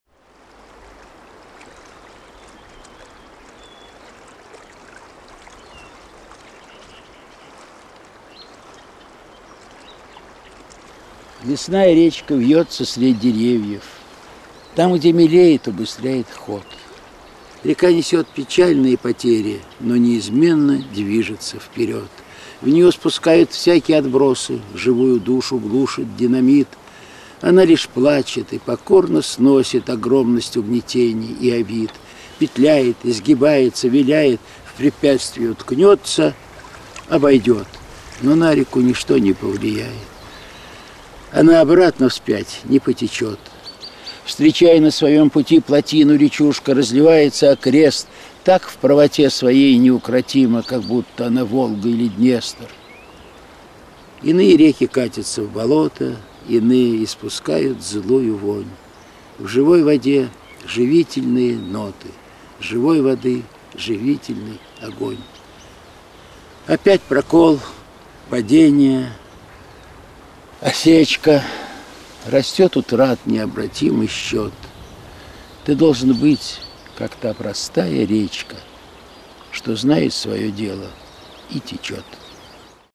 Эльдар Рязанов. Лесная речка вьется средь деревьев...
Читает автор.
Лесная речка вьется средь деревьев...
Там, где мелеет, убыстряет ход.
Река несет печальные потери,
Но неизменно движется вперед...